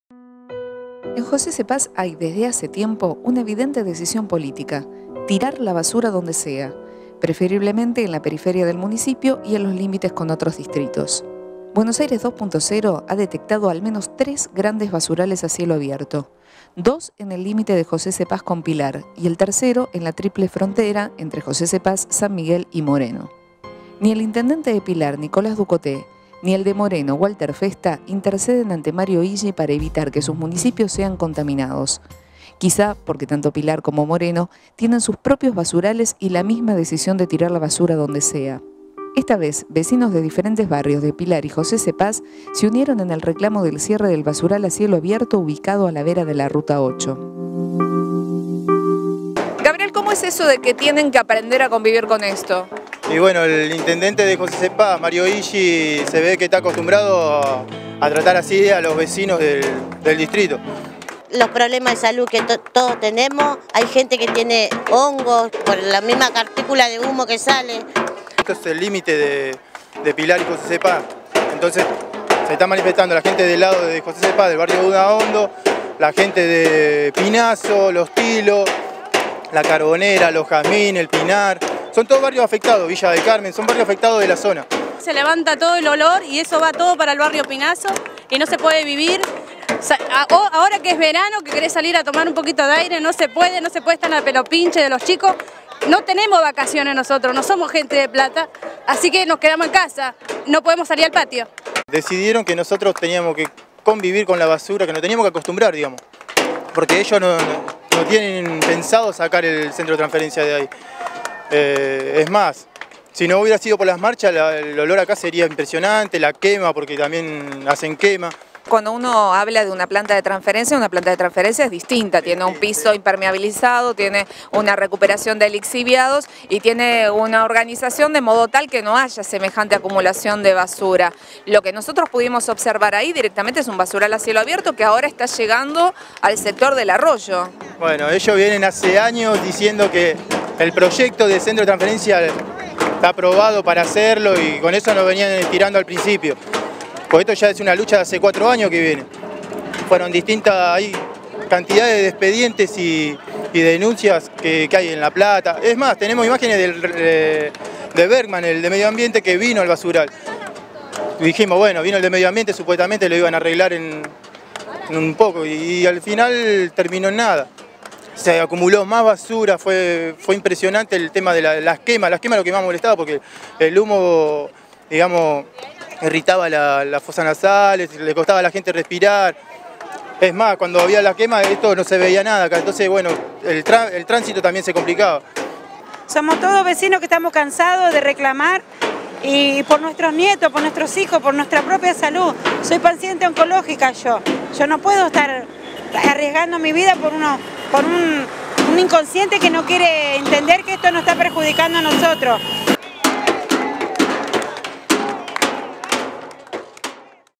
Mario Ishii y el arte de esconder la basura bajo el poncho
José C. Paz ha decidido disponer los residuos domiciliarios donde sea.Ésta decisión afecta la vida y salud de miles de vecinos perjudicados por la gran cantidad de basurales clandestinos del distrito.